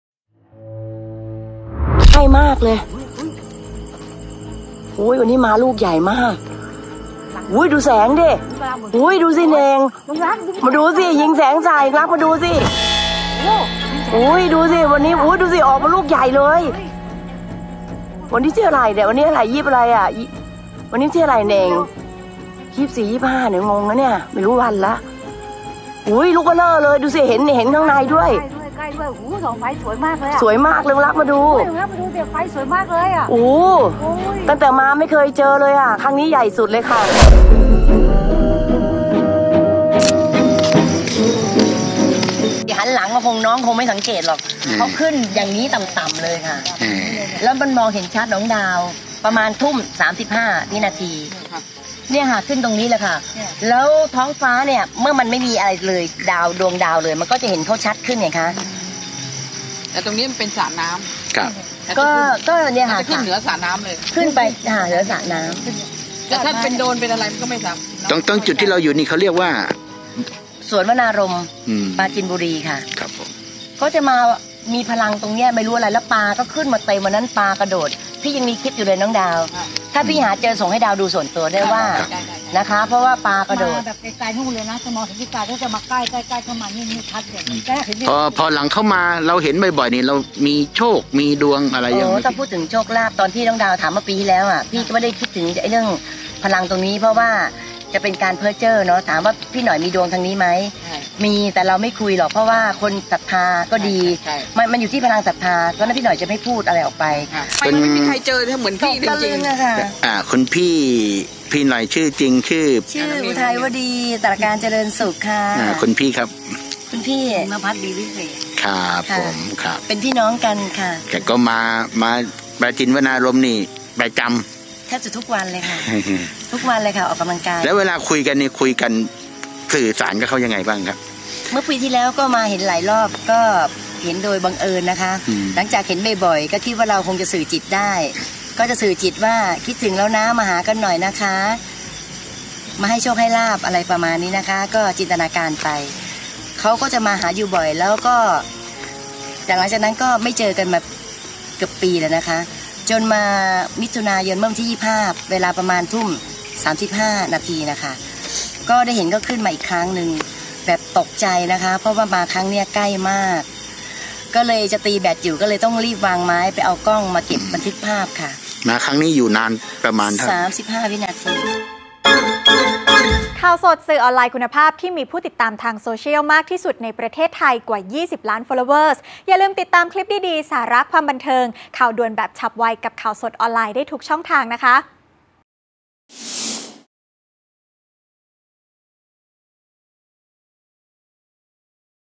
สองพี่น้อง เห็นคาตาเชื่อ ยูเอฟโอ บุก วัตถุปริศนาเหนือปราจีนบุรี ปล่อยลำแสงทักทาย: Khaosod - ข่าวสด
อดีตผู้สมัคร สส.พบยาน UFO.ลำใหญ่ บินมาดูดพลังงานเหนือสวนสาธารณะปราจีนวนารมณ์พร้อมยิงแสงทักทายระบุปีนี้พบครั้งแรกเชื่อว่ามาทักทายหลังจากเคยพบครั้งแรกเมื่อปลายเดือน มิ.ย.ปีที่ผ่านมา